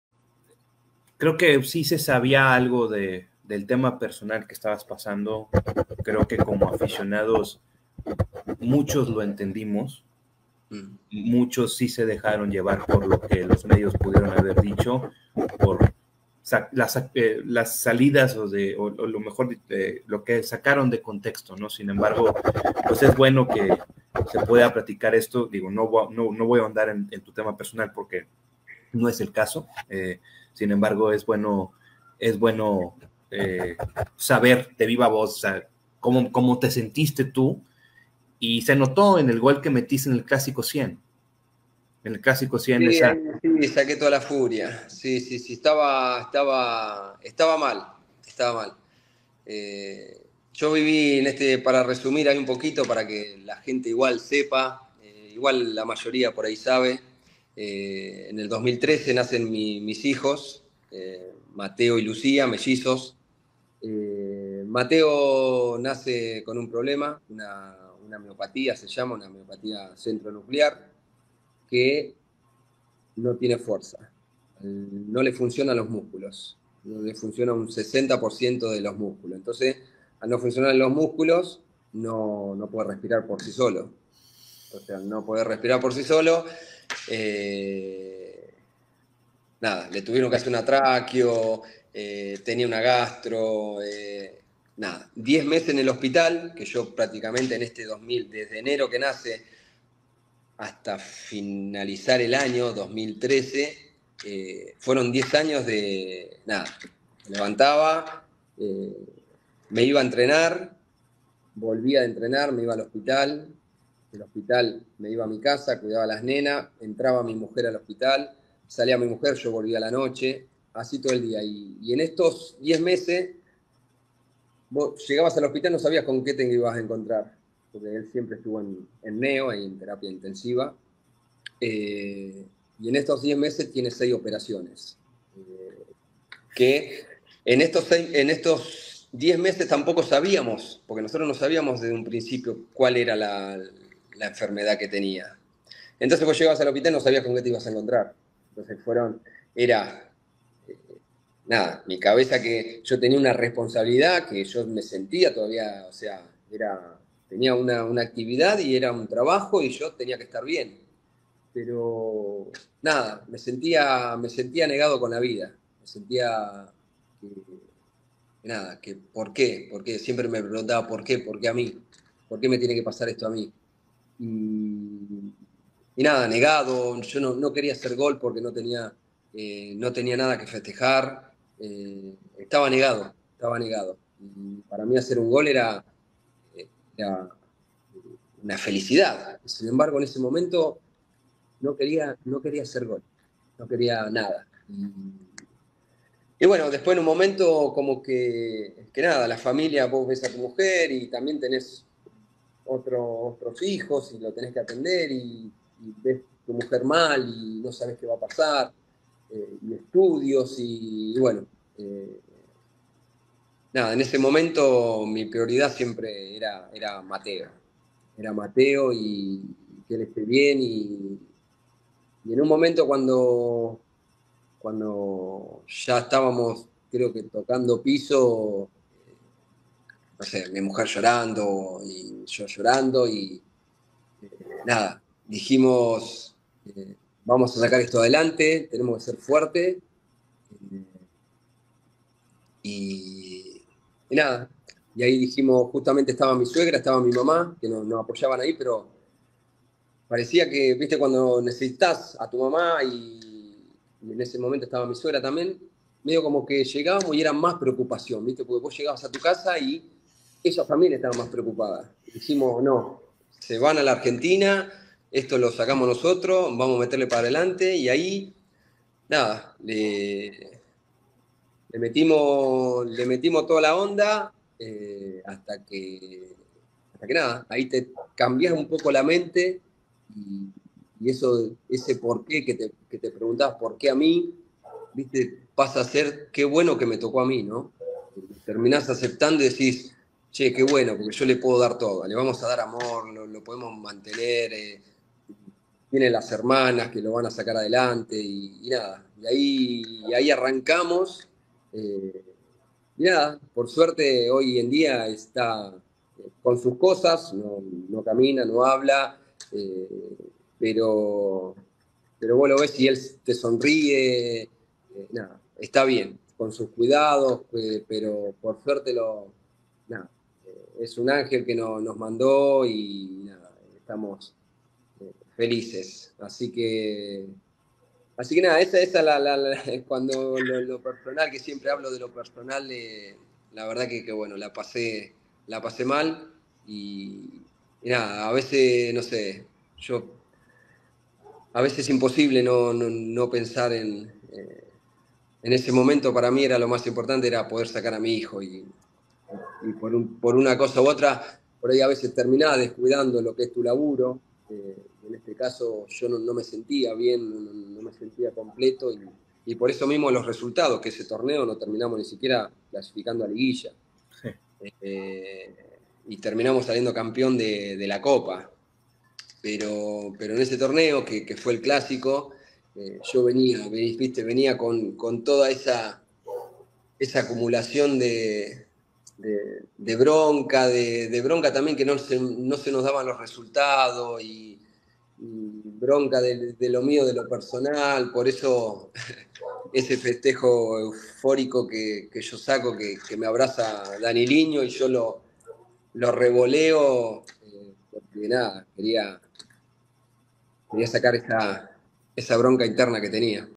Lucas Lobos. Solo Tigres #83 | La HISTORIA detrás del FESTEJO en el CLÁSICO 100
Lucas Lobos nos cuenta cómo fue su sentimiento por disputar el clásico número 100 y el enfrentar adversidades